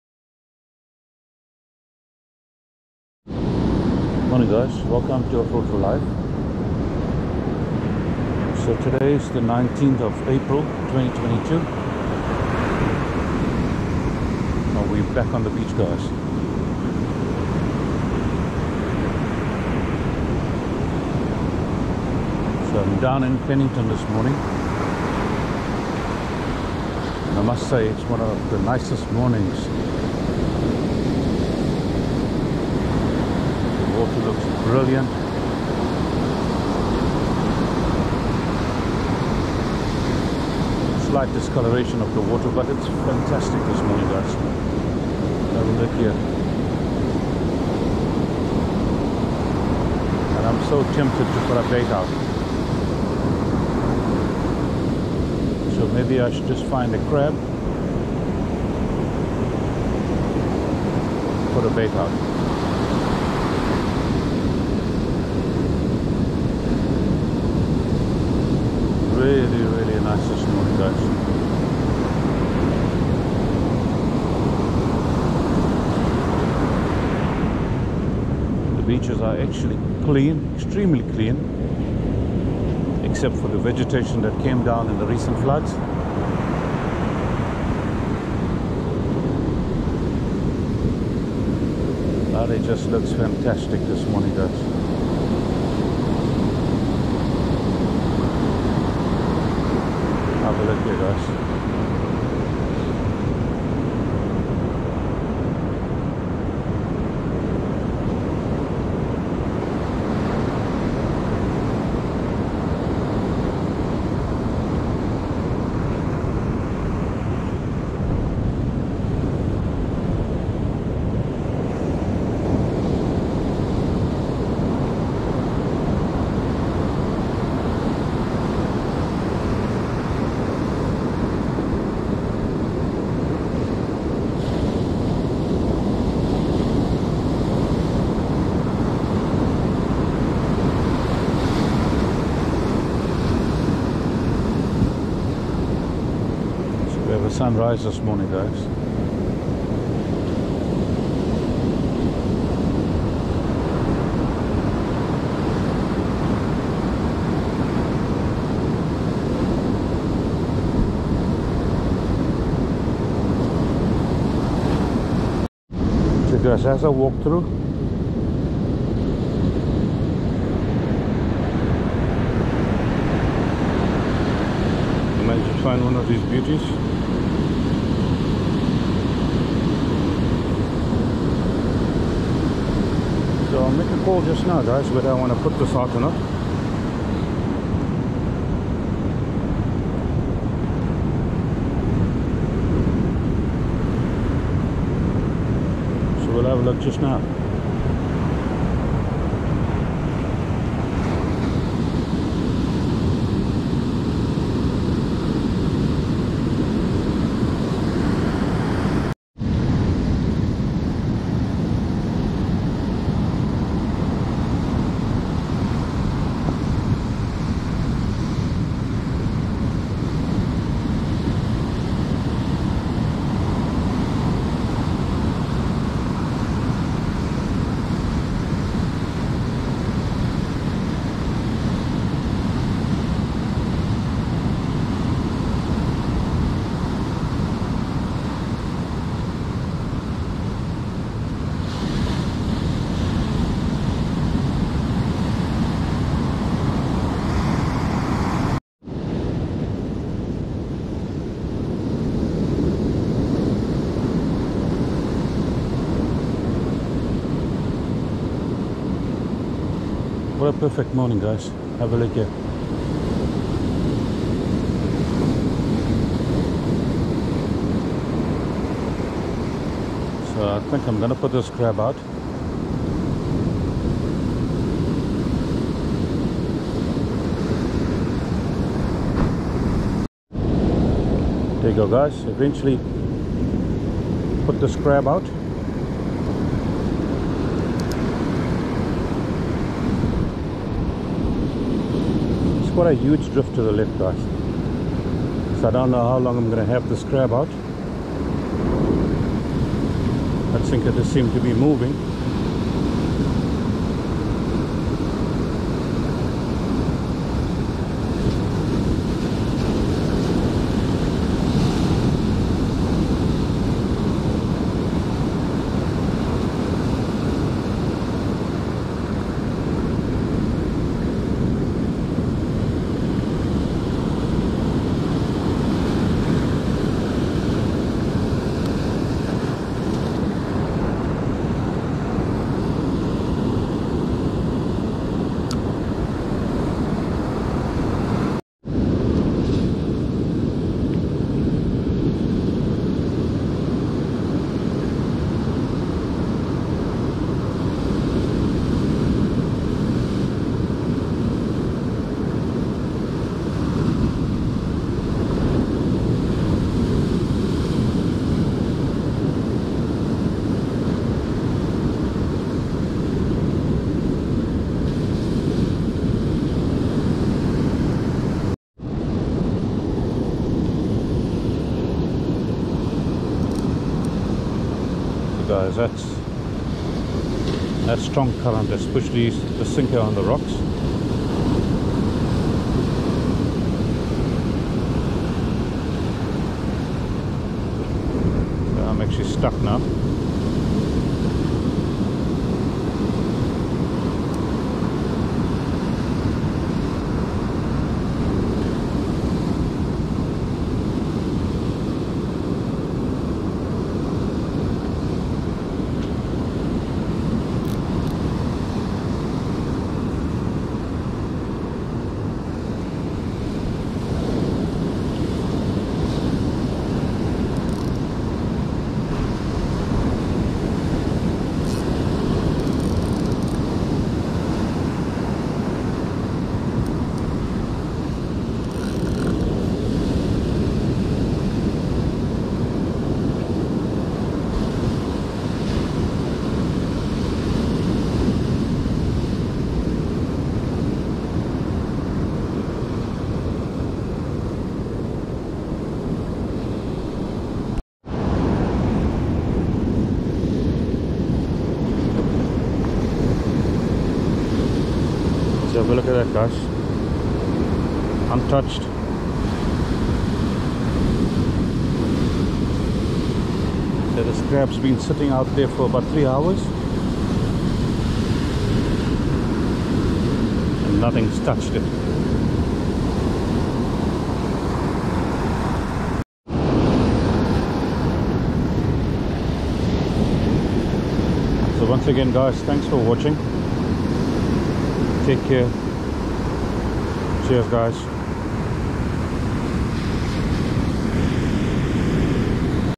OFFROAD4LIFE, Flooding, Fishing after ths floods, Pennington, 19.04.2022
OFFROAD4LIFE, Fishing is the life, Pennington, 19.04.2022